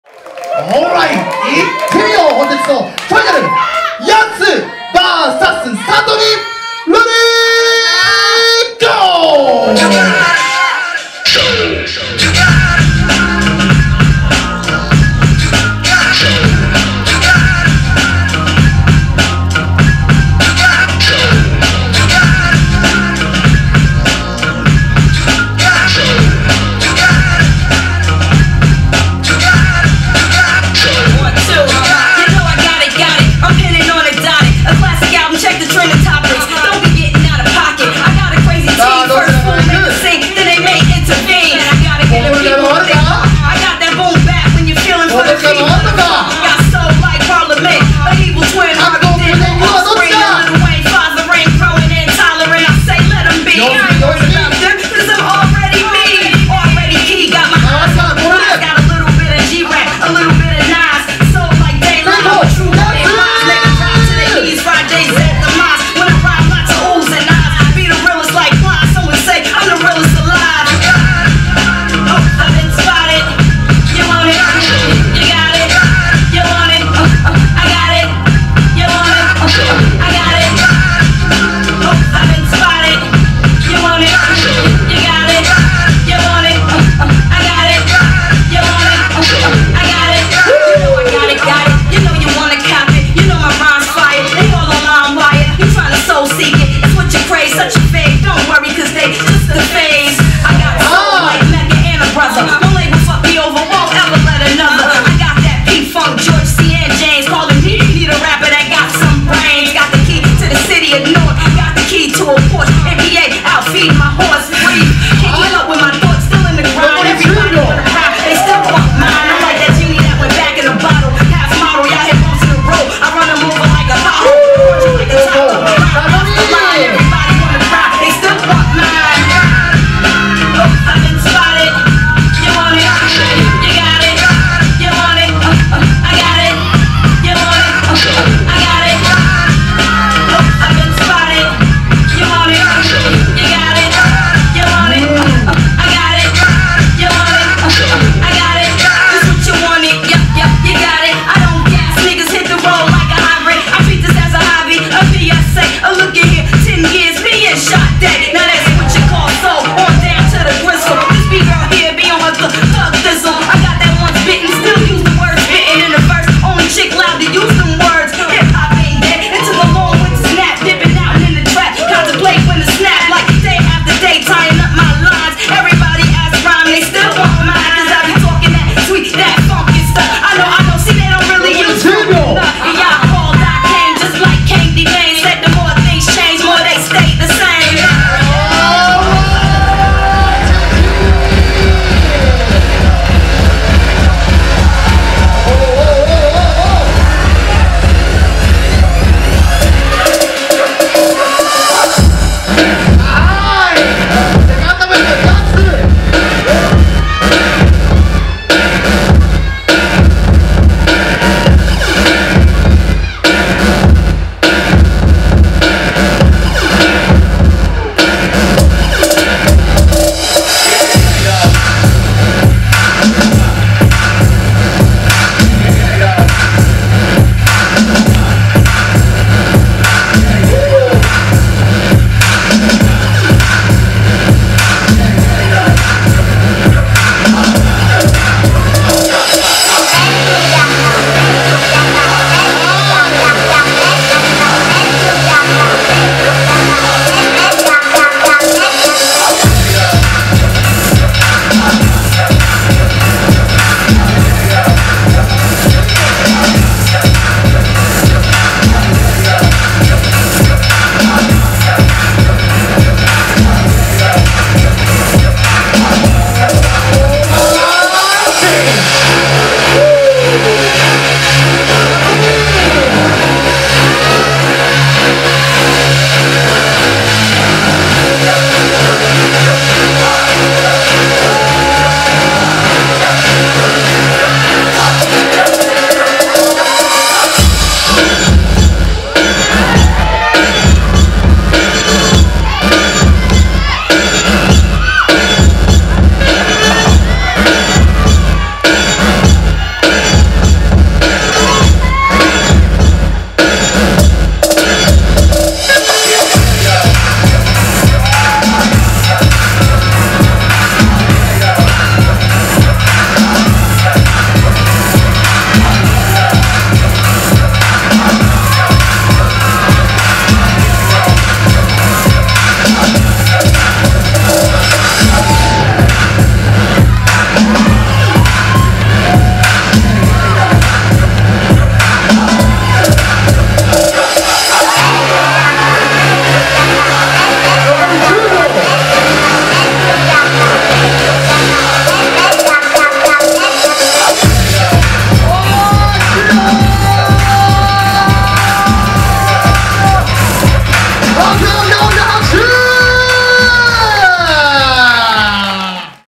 YASS(Beat Buddy Boi) vs SATOMI(L.O.F.D) DANCE@LIVE 2014 HIPHOP KANTO vol.5【FINAL】
WIN :  SATOMI (L.O.F.D)
Street dance Battle "DANCE@LIVE" in Japan.

jan. 21 2014 DANCE@LIVE 2014 HIPHOP KANTO vol.5

ダンスのニュースを網羅するストリートダンスポータルサイト Dews

日本から世界へ感動を届けるエンターテイメント会社 ANOMALY INC